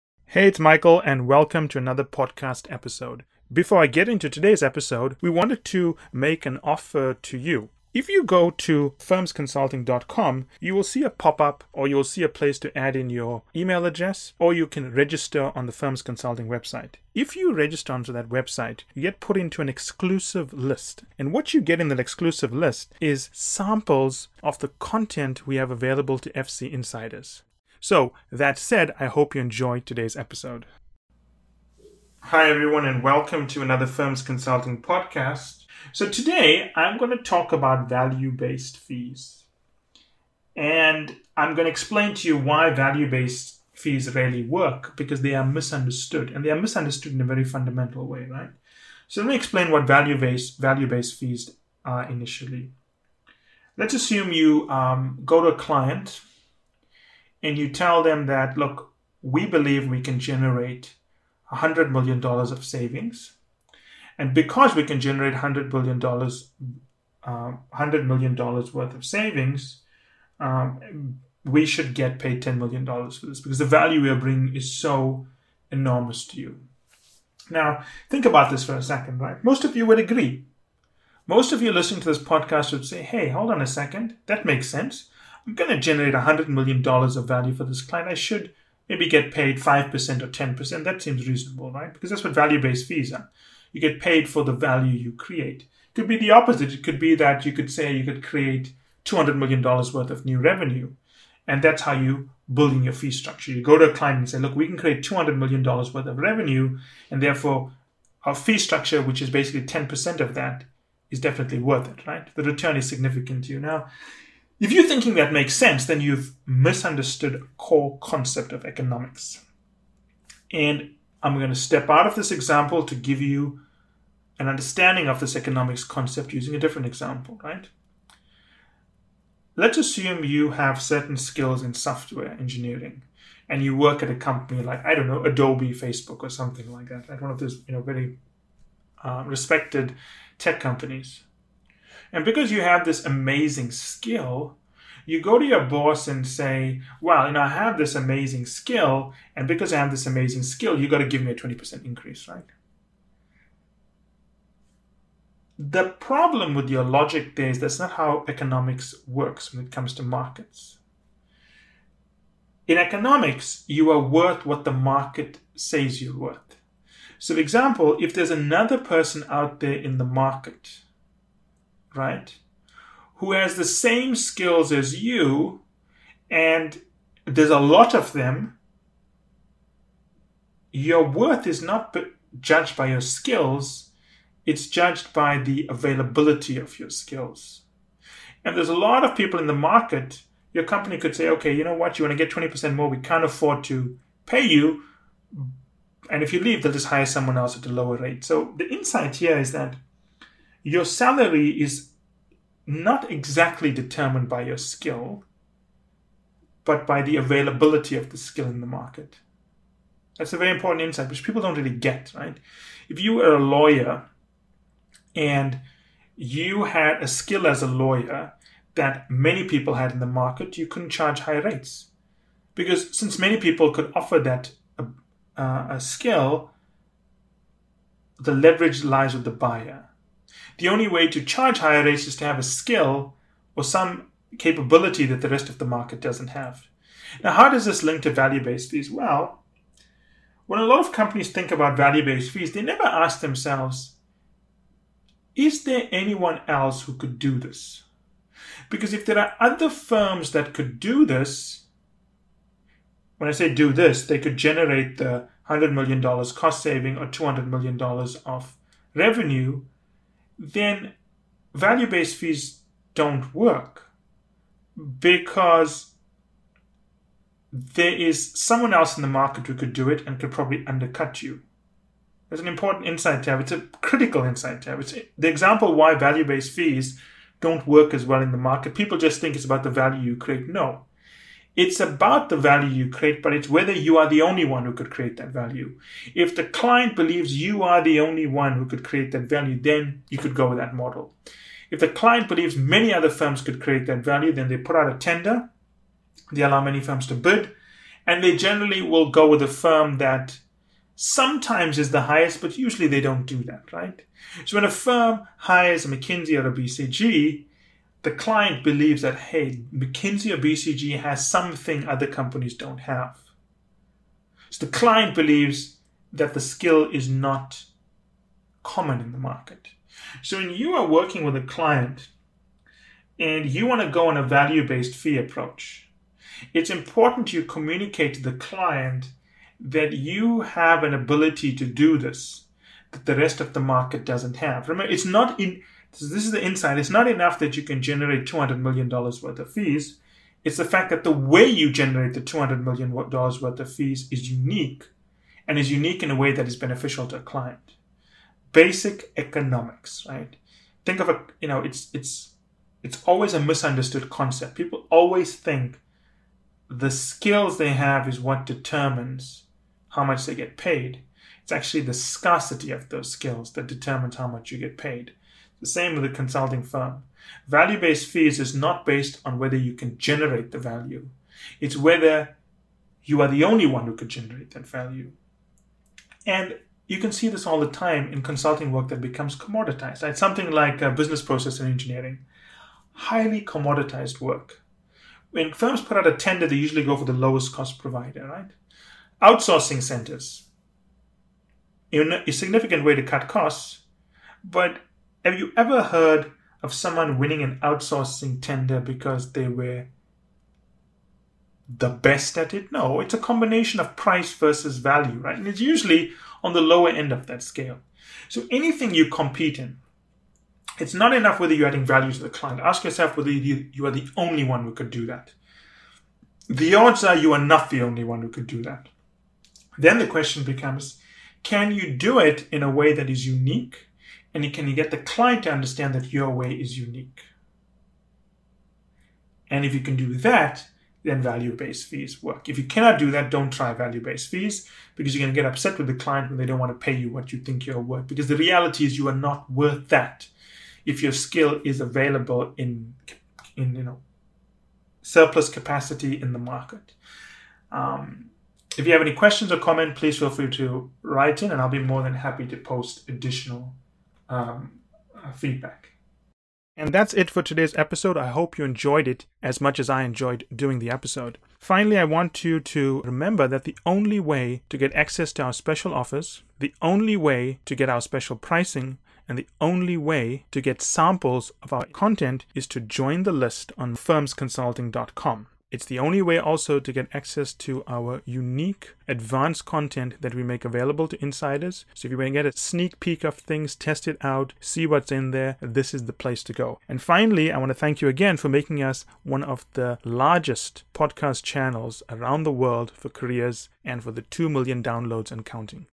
Consulting Fees | Why Value Based Fees Rarely Work in Management Consulting | Consulting Business
1.00 - What is value based fees.
2.08 - Working at a software company example. Explaining how economics works, how what you are worth is determined. Lawyer example.
4.03 - What companies do not ask themselves when thinking about value-based fees.
5.07 - What happens when someone hires McKisney or BCG.
5.51 - Is skill that you have determines how much you are paid? If not, then what determines how much you are paid?
6.18 - Commoditized consulting work.
6.52 - What should you ask yourself when serving a client. Can you do work in a way that is unique and can you get client understand that your way is unique?